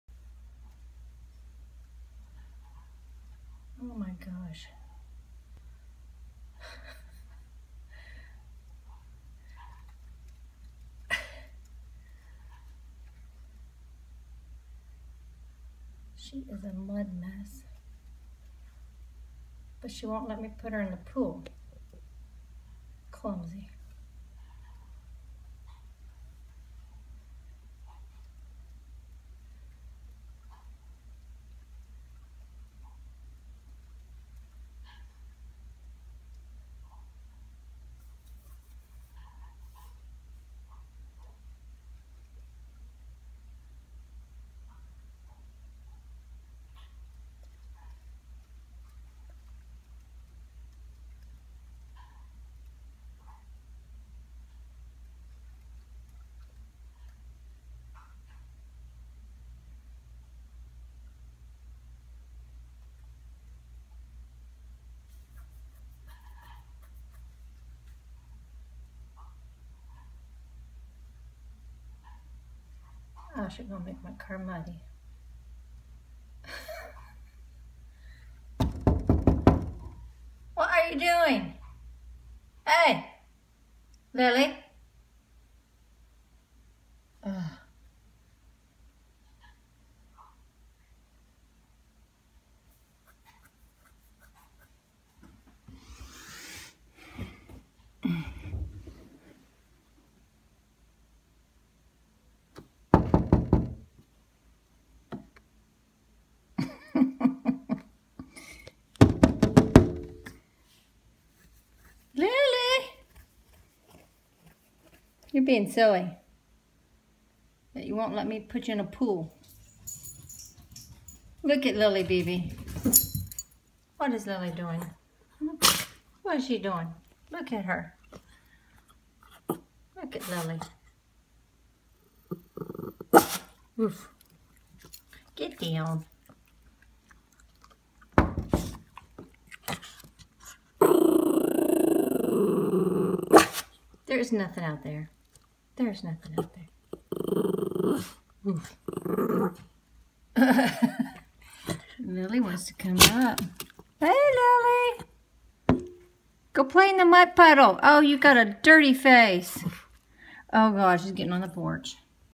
Joys of a rainy day for a dog
It's funny to see how pups entertain themselves